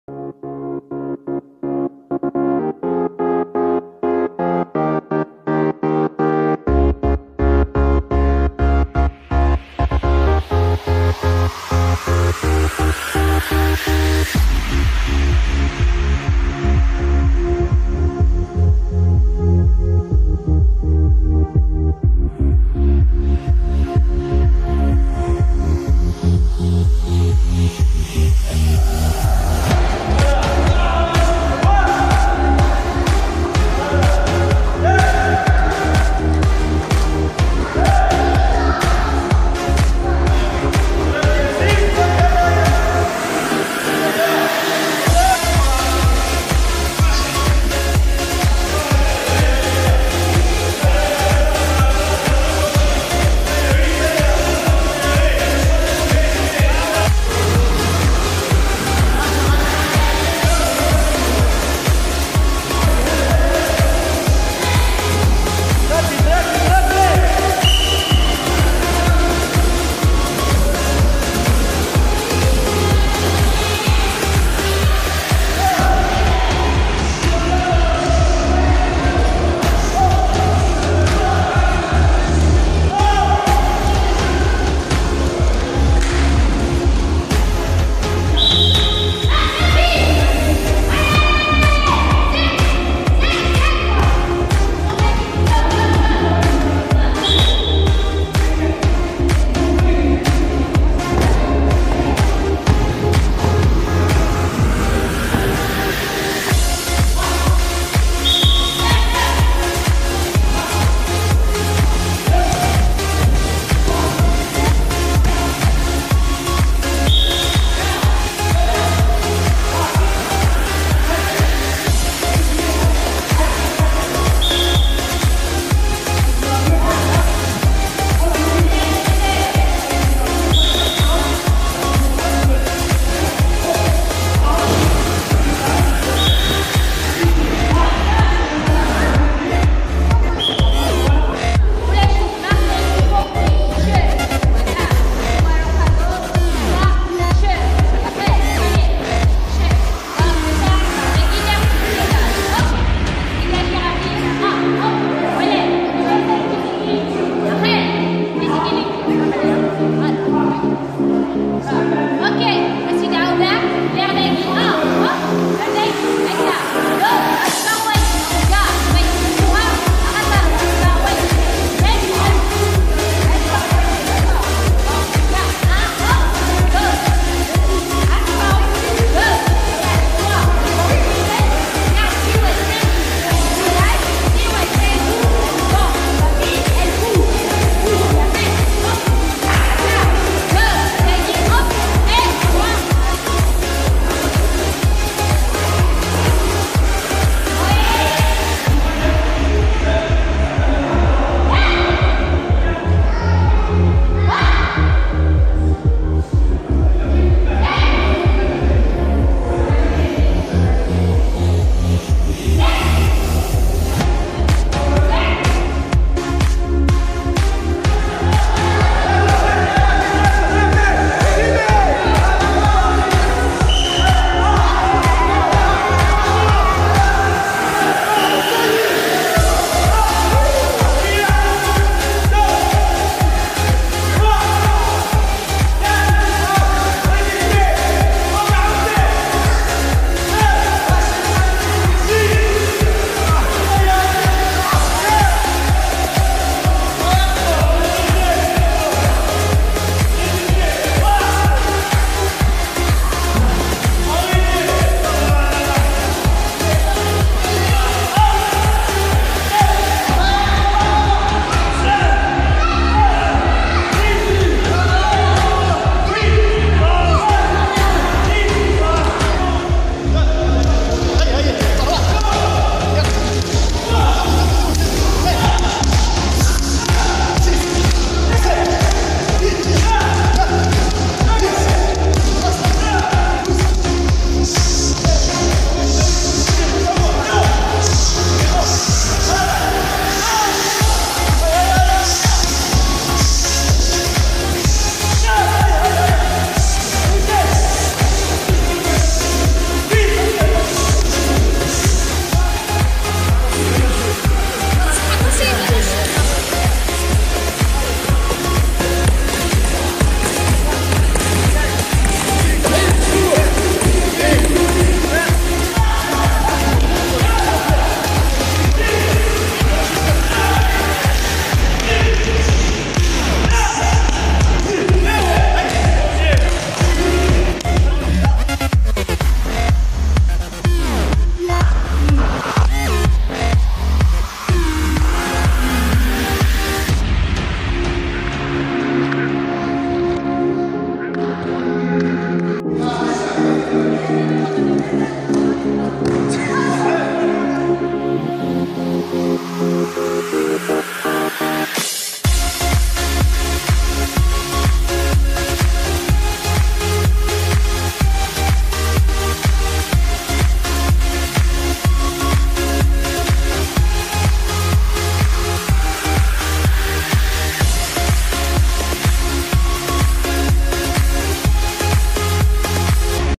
ESMEKLA entraînements physique VO-Vietnam
La volonté, la persévérance et la discipline.
des caractères que le club ESMEKLA possède.
avec leur entraîneur cheikh RAFIK Bentaha les athlètes de ESMEKLA ce donnent toujours a fond.
cheikh Rafik est fière d'avoir une équipe toujours prête a ce surpasser :
- Mesloub Nassima épse Bentaha.
- Mesloub Massinissa.
- Matouk Salim.
- Inouri mokrane.
- Boussaid Mohand.
- Hamidane Thiziri.
- Zerraf Nassima.
- lydia
- et les 200 autres athlètes.